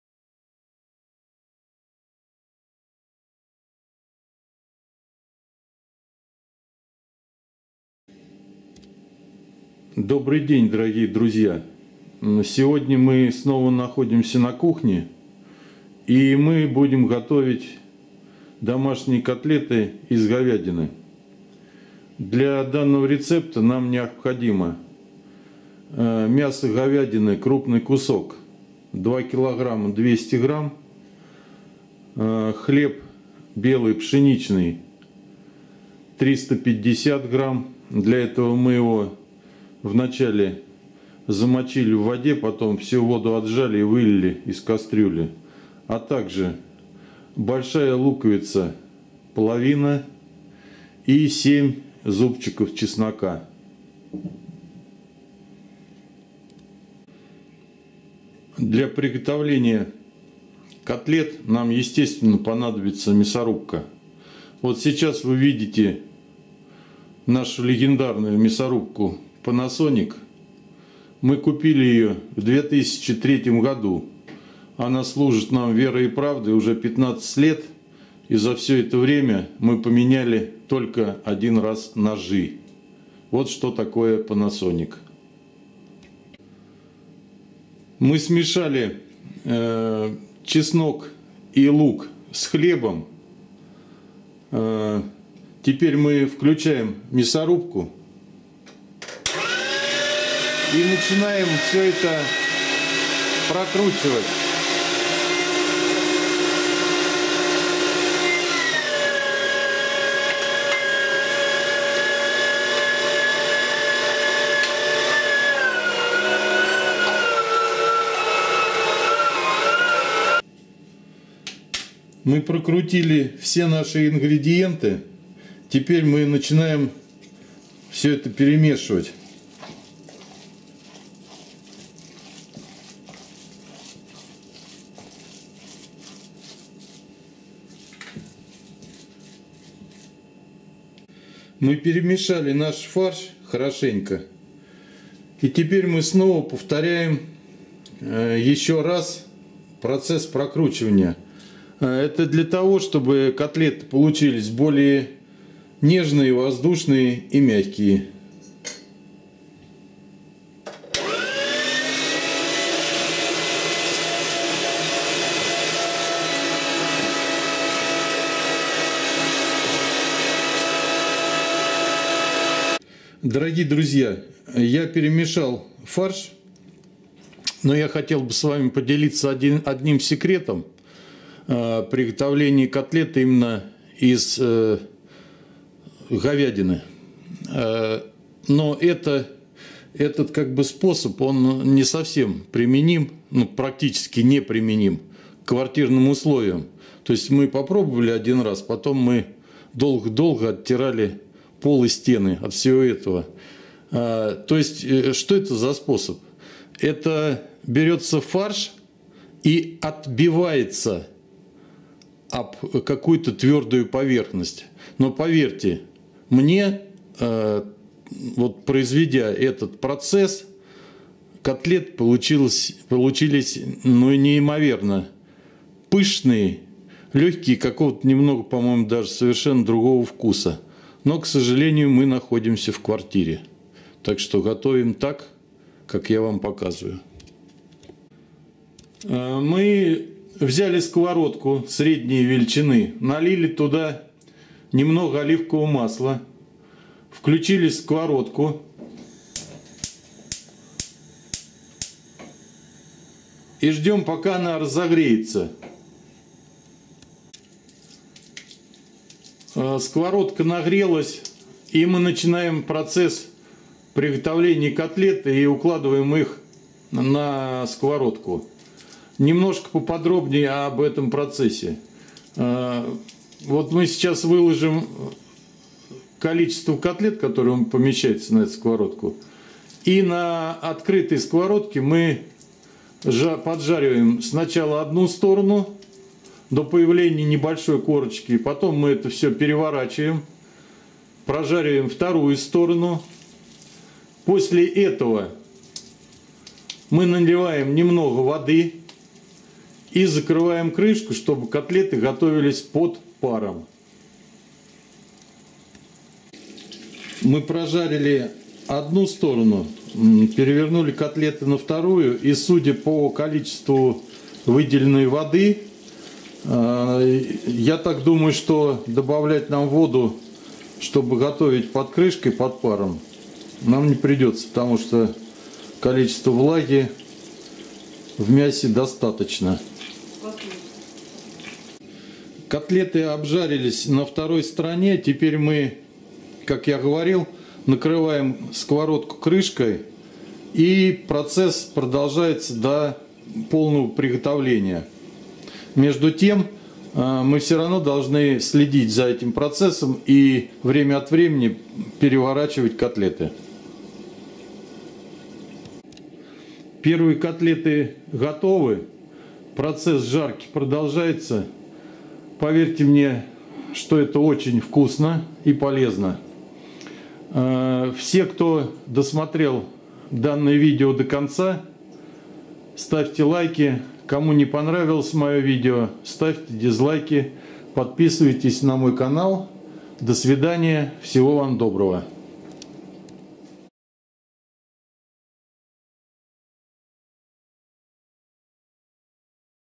Домашние котлеты из говядины
В данном сюжете мы готовим домашние котлеты из говядины.